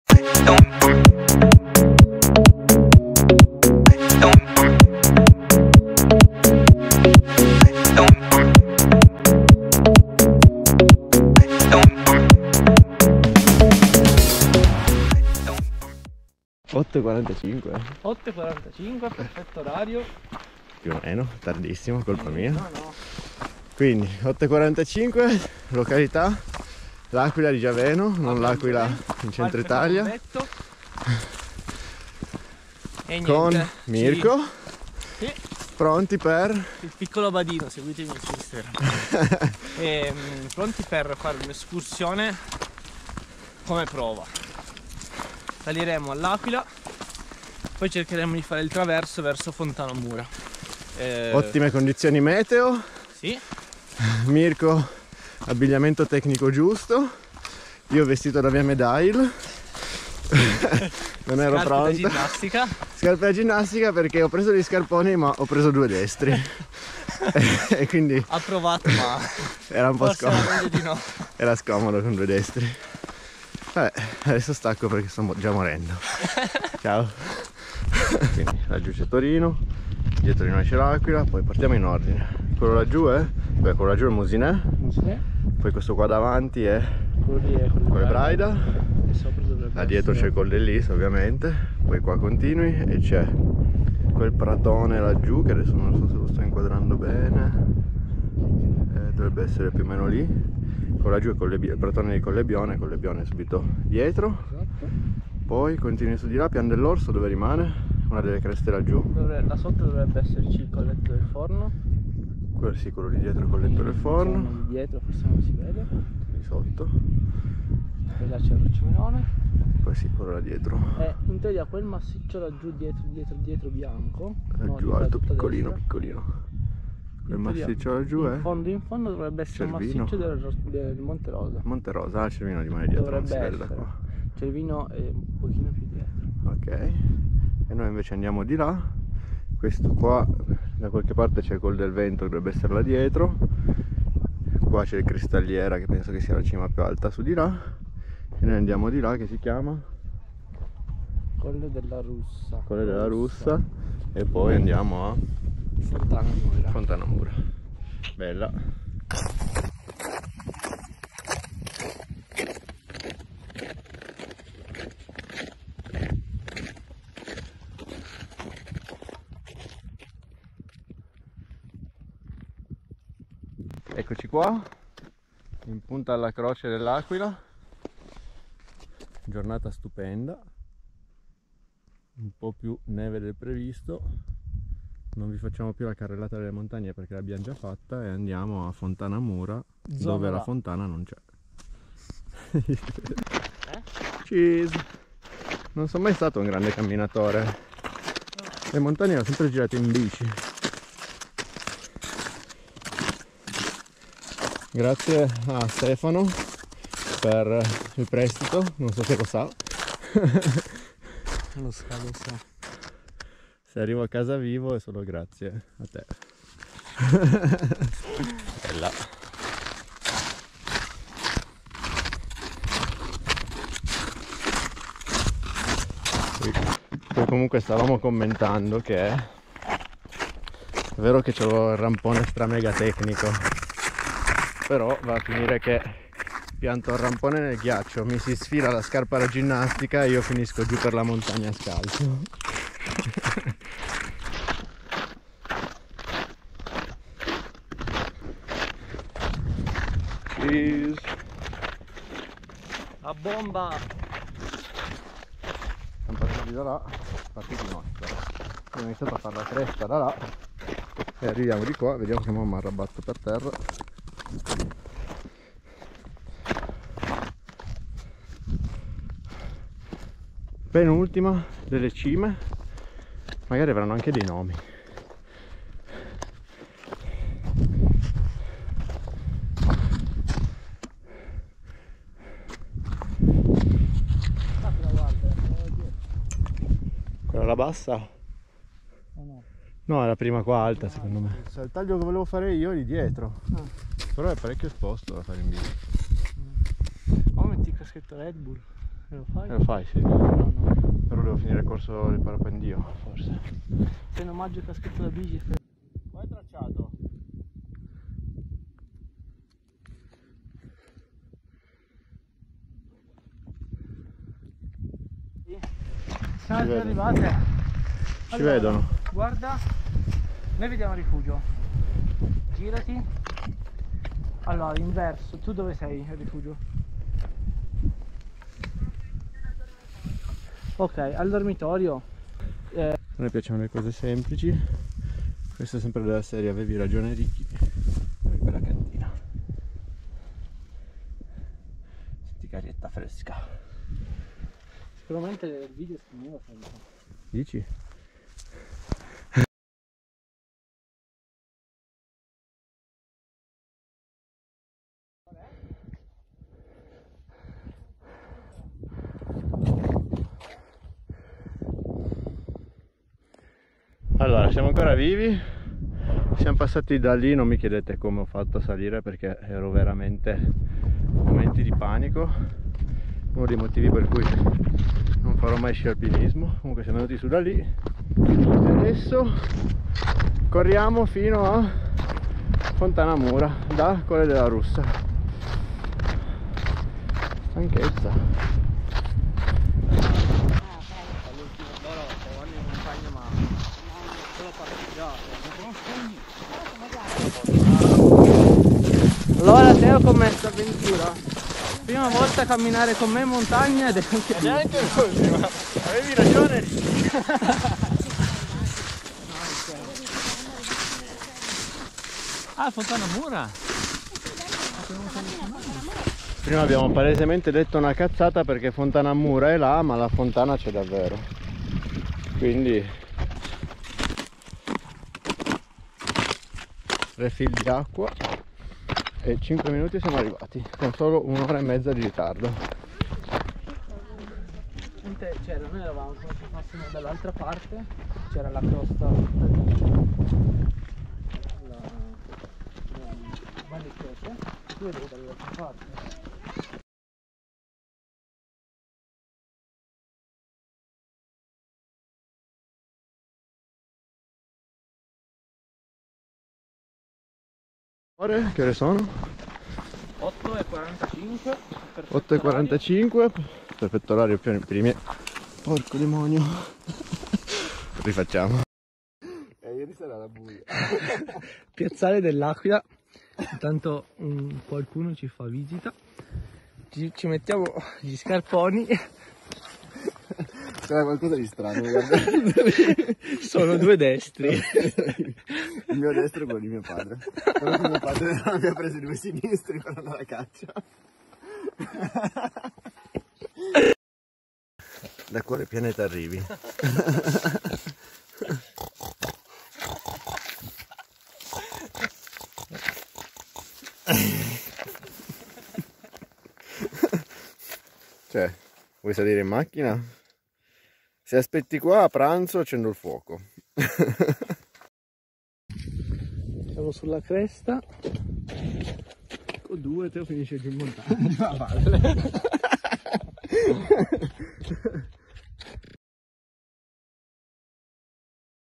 Passeggiata sulle creste de L'Aquila di Giaveno
Passeggiata sulle creste de L'Aquila di Giaveno in direzione Rifugio Fontana Mura.

Buona visione!